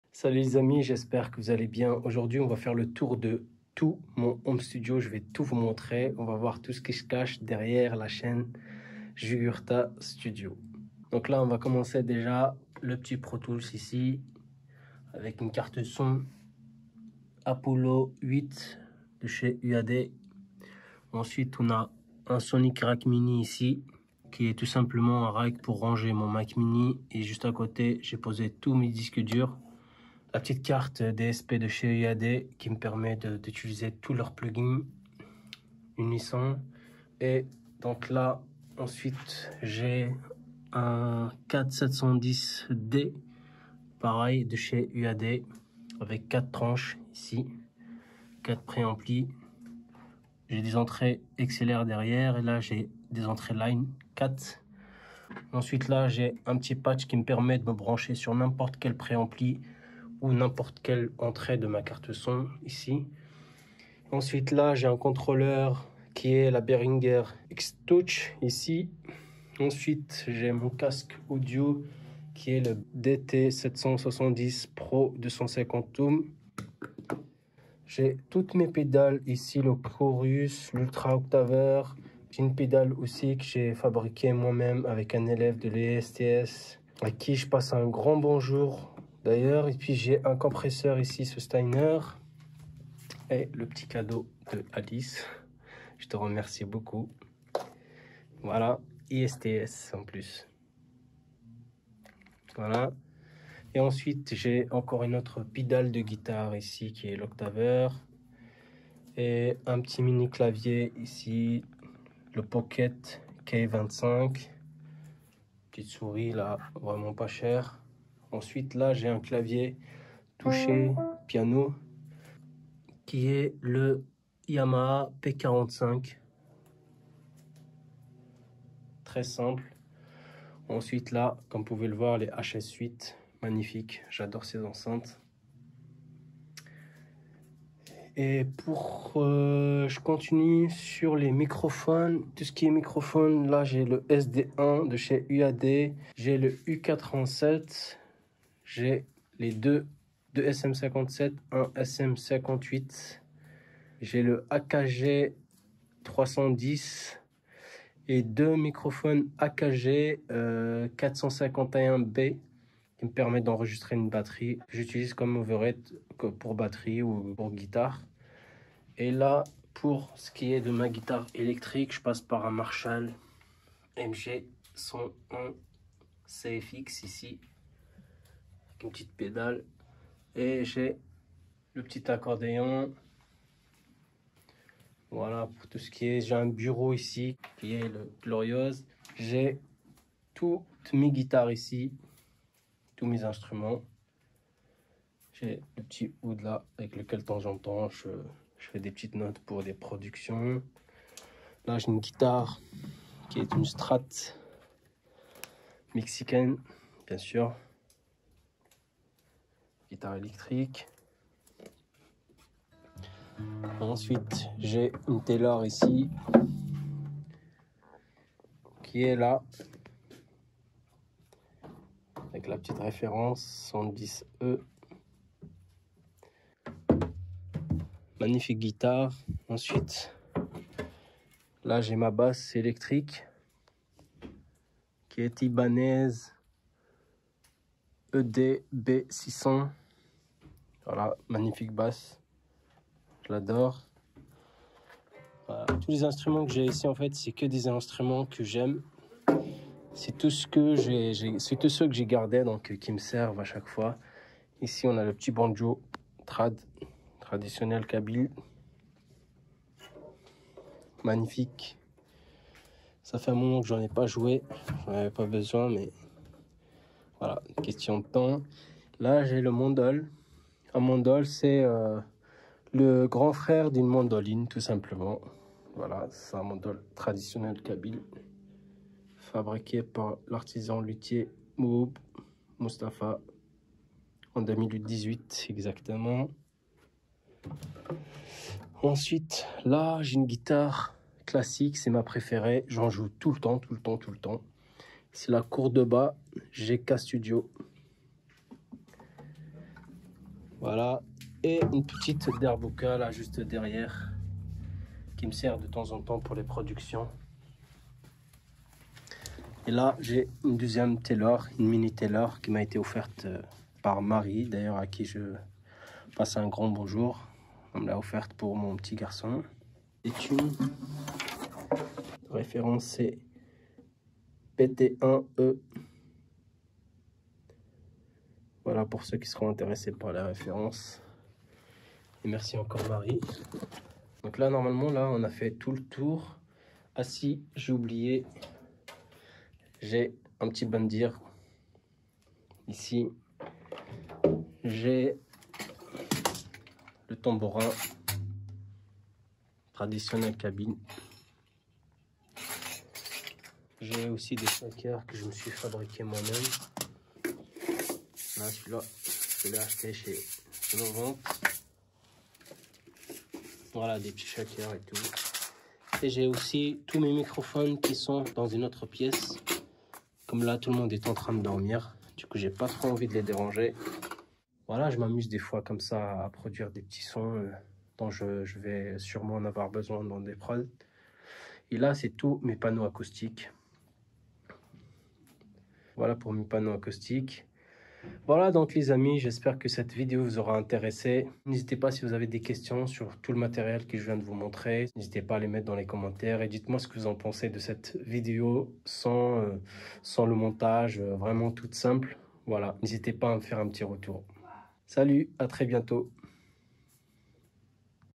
Mon Home Studio : Tout Ce Que J'utilise pour Produire et Mixer
Découvrez Tout Mon Matériel de Production.

▬▬▬▬▬▬▬▬▬▬▬ A PROPOS DE MES VIDÉO  ▬▬▬▬▬▬▬▬▬▬▬
Tous ce que tu vas trouver sur ma chaîne Youtube JugurthaStudio